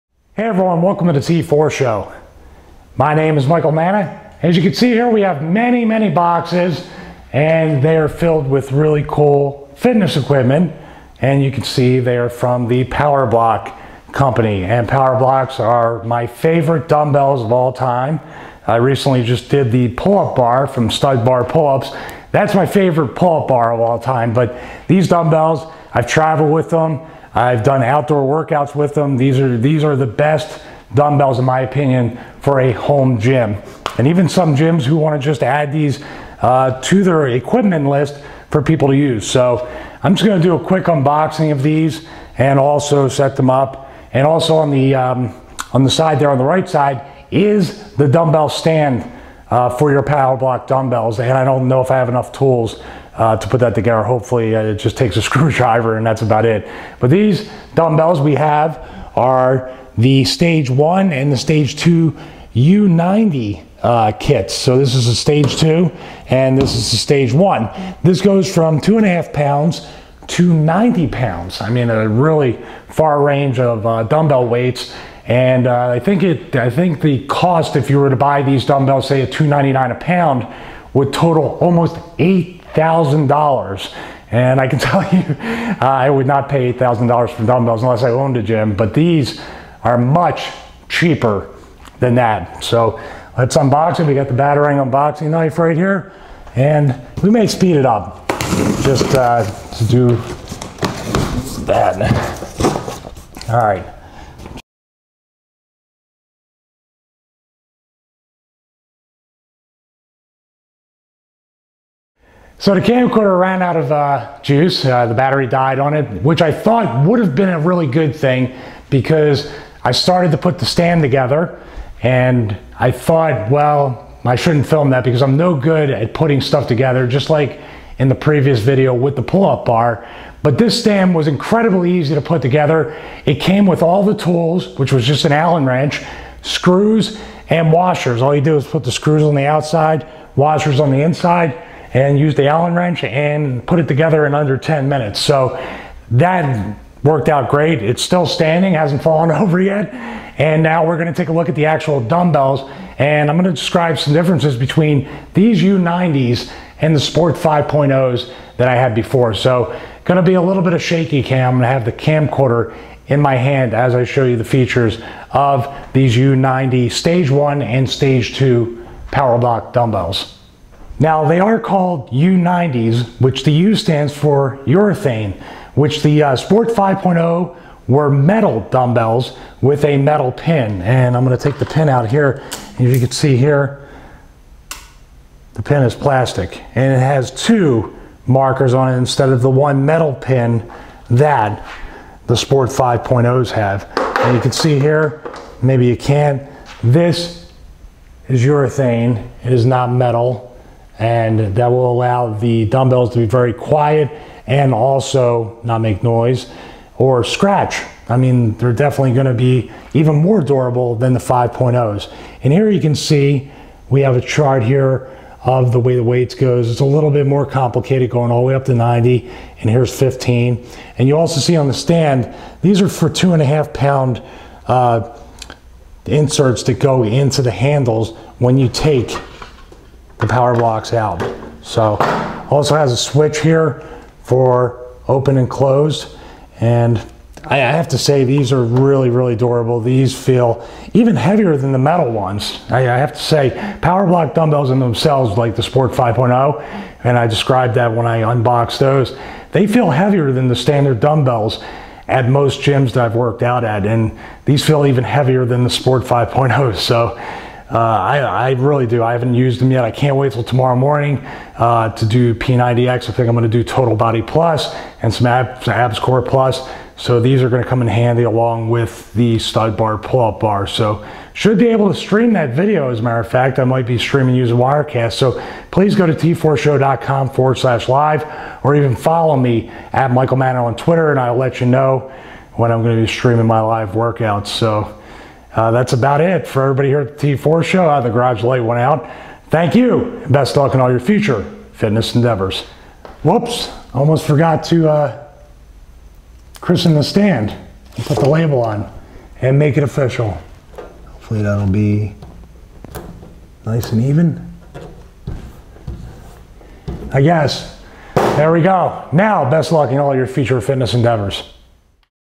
Powerblock U-90 Stage 1 and Stage 2 Kits
I unbox and take a tour of the features of the Powerblock U-90 Stage 1 and Stage 2 Kits